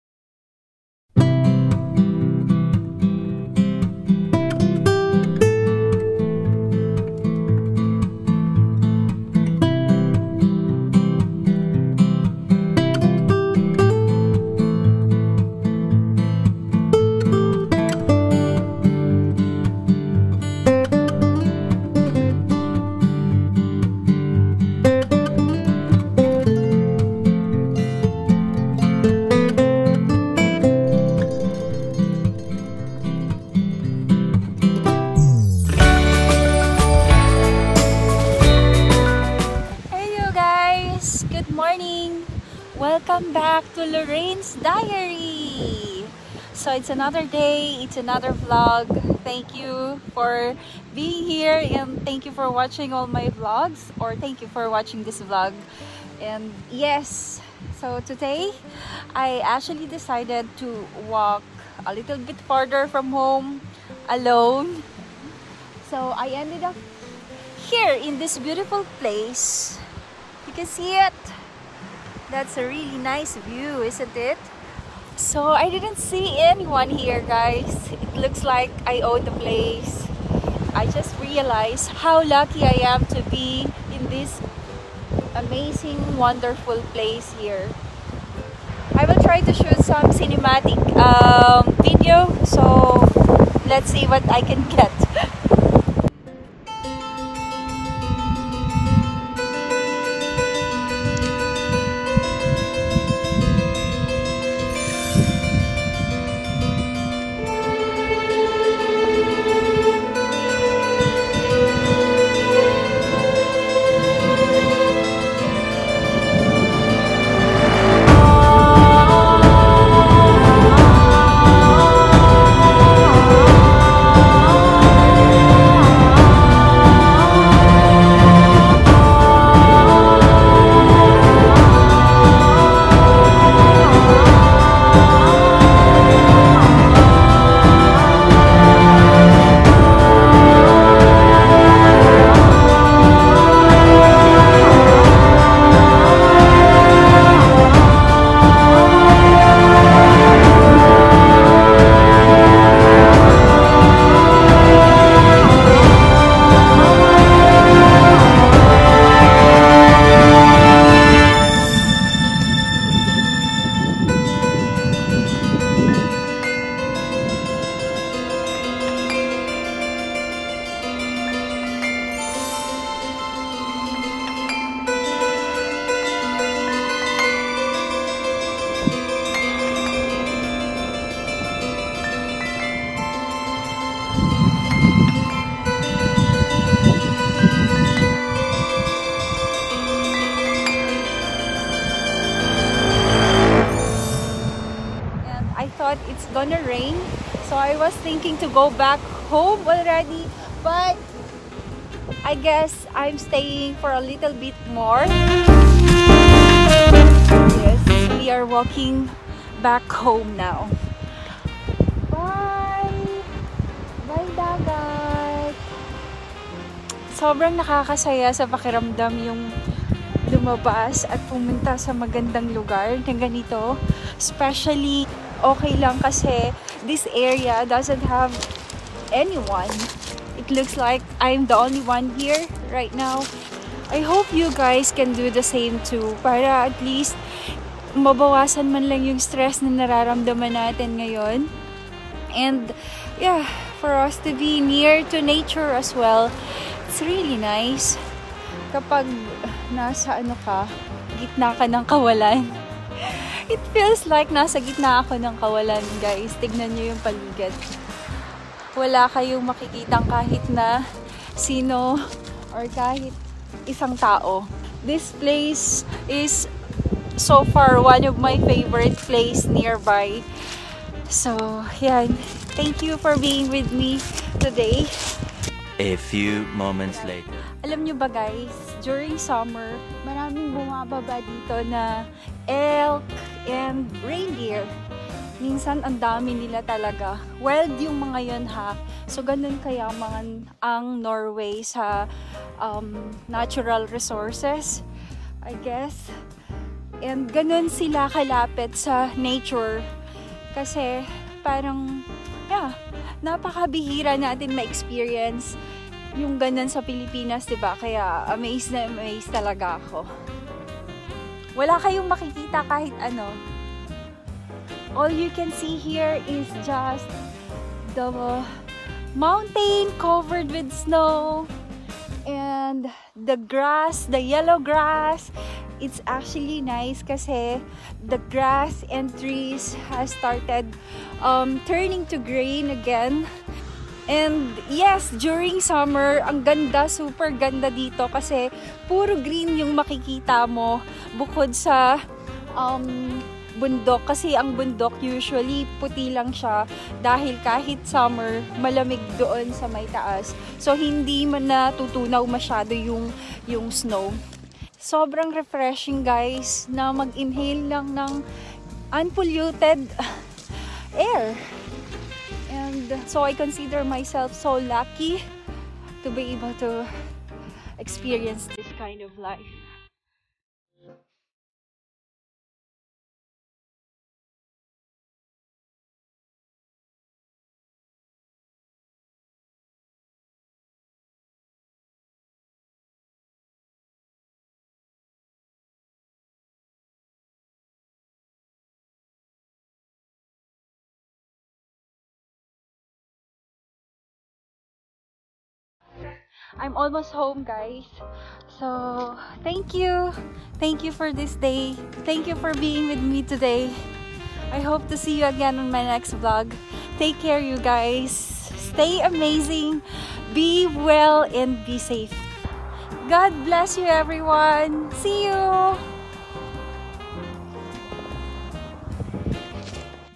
NORWAY VLOGS || NAMASYAL SA TABING DAGAT
Hi Guys,

Welcome to my Diary, I am Loraine.  Another beautiful scenery of Norway will be shown in this vlog. And though I forgot to mention in the vlog, this place is one of the place where the vikings has have lived many years ago so according to some there are a lot of their things were buried in this place. Don´t forget to like it.